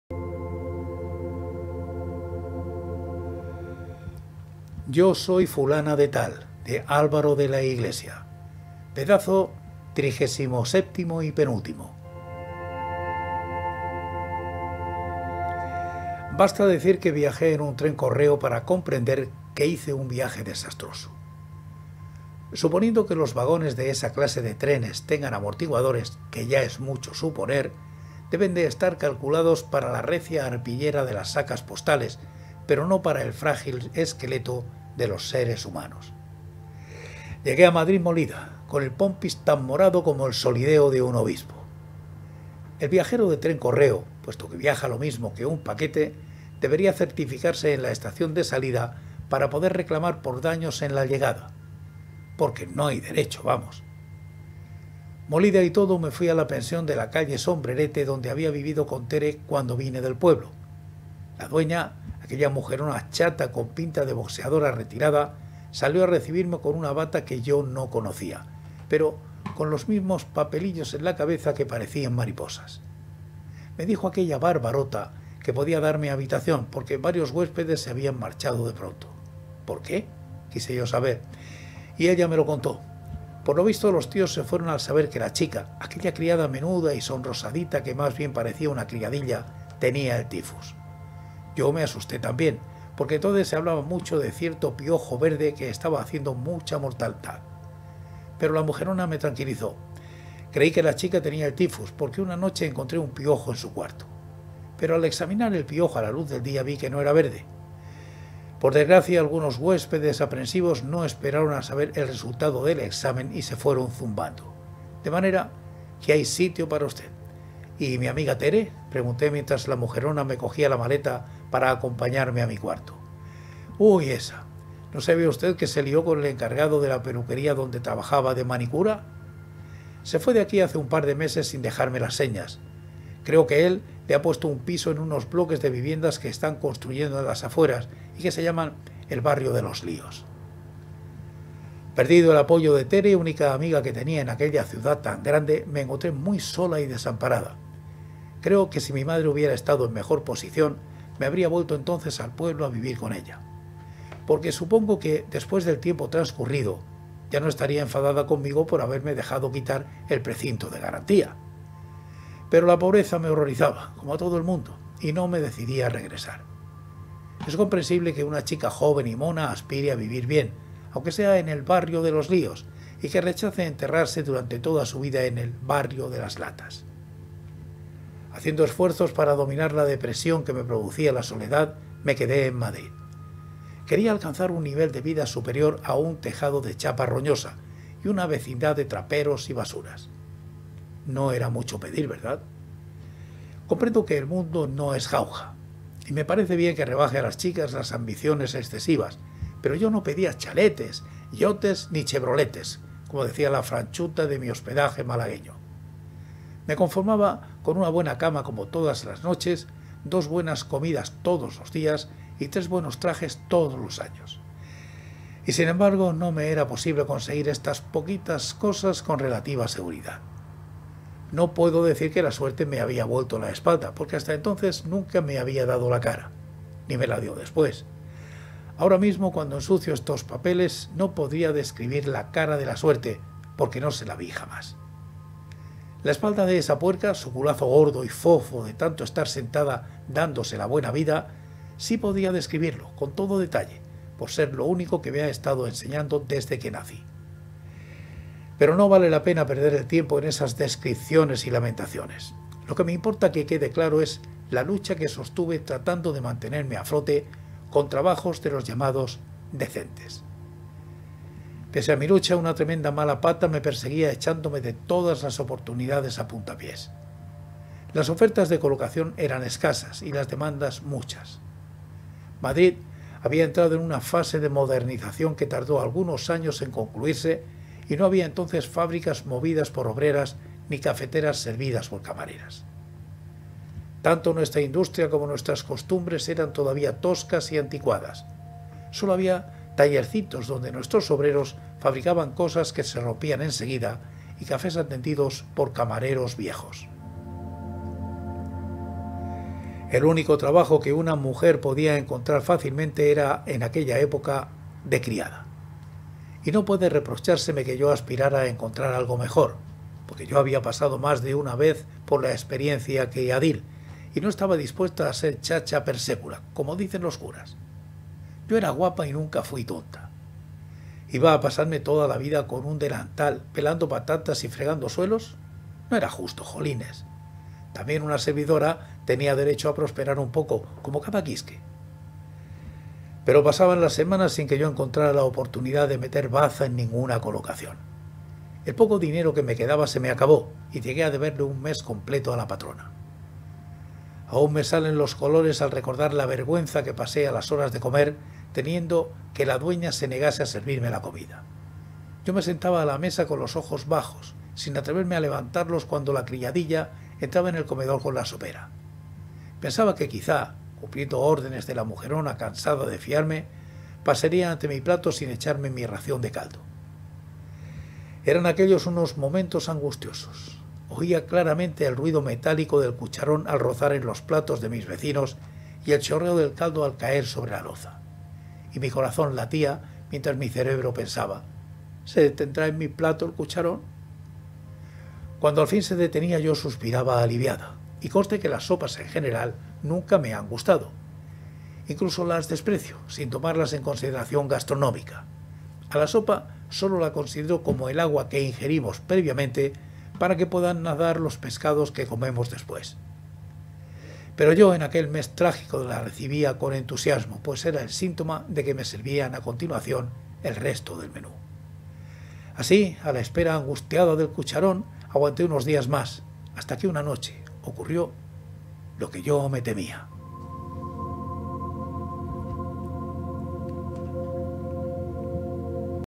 Yo soy fulana de tal, de Álvaro de Laiglesia - 37 - Pedazo 37 - La Voz Silenciosa
🚂Basta decir que viajé en un tren correo para comprender que hice un viaje desastroso. Suponiendo que los vagones de esa clase de trenes tengan amortiguadores, que ya es mucho suponer, deben de estar calculados para la recia arpillera de las sacas postales, pero no para el frágil esqueleto de los seres humanos.

💖 HAZ CLICK EN EL BOTÓN "UNIRME" y apoya a este canal.
SEGURO QUE TE INTERESA ESTE AUDIOLIBRO:
Yo soy fulana de tal, de Álvaro de Laiglesia.

Pedazo 37

Llegué a Madrid molida, con el pompis tan morado como el solideo de un obispo. El viajero de tren correo, puesto que viaja lo mismo que un paquete, debería certificarse en la estación de salida para poder reclamar por daños en la de llegada. Porque no hay derecho, vamos.

Molida y todo, me fui a la pensión de la calle Sombrerete, donde había vivido con Tere cuando vine del pueblo. La dueña, aquella mujerona chata con pinta de boxeadora retirada, salió a recibirme con una bata que yo no conocía; pero con los mismos papelillos en la cabeza que parecían mariposas.

Me dijo aquella barbarota que podía darme habitación, porque varios huéspedes se habían marchado de pronto.

—¿Por qué? —quise yo saber.

Y ella me lo contó. Por lo visto, los tíos se fueron al saber que la chica —aquella criada menuda y sonrosadita que más bien parecía una criadilla— tenía el tifus. Yo me asusté también, porque entonces se hablaba mucho de cierto piojo verde que estaba haciendo mucha mortandad. Pero la mujerona me tranquilizó:

—Creí que la chica tenía el tifus, porque una noche encontré un piojo en su cuarto. Pero al examinar el piojo a la luz del día, vi que no era verde. Por desgracia, algunos huéspedes aprensivos no esperaron a saber el resultado del examen, y se fueron zumbando. De manera que hay sitio para usted.

—¿Y mi amiga Tere? —pregunté, mientras la mujerona me cogía la maleta para acompañarme a mi cuarto.

—¡Huy ésa! ¿No sabía usted que se lió con el encargado de la peluquería donde trabajaba de manicura? Se fue de aquí hace un par de meses sin dejarme las señas. Creo que él le ha puesto un piso en unos bloques de viviendas que están construyendo en las afueras, y que se llaman «el barrio de los líos».

Perdido el apoyo de Tere, única amiga que tenía en aquella ciudad tan grande, me encontré muy sola y desamparada. Creo que si mi madre hubiera estado en mejor posición, me habría vuelto entonces al pueblo a vivir con ella. Porque supongo que, después del tiempo transcurrido, ya no estaría enfadada conmigo por haberme dejado quitar el precinto de garantía. Pero la pobreza me horrorizaba, como a todo el mundo, y no me decidí a regresar. Es comprensible que una chica joven y mona aspire a vivir bien, aunque sea en «el barrio de los líos», y que rechace enterrarse durante toda su vida en «el barrio de las latas».

Haciendo esfuerzos para dominar la depresión que me producía la soledad, me quedé en Madrid. Quería alcanzar un nivel de vida superior a un tejado de chapa roñosa, y una vecindad de traperos y basuras.

No era mucho pedir, ¿verdad?

José Fco. Díaz-Salado en su programa de LA VOZ SILENCIOSA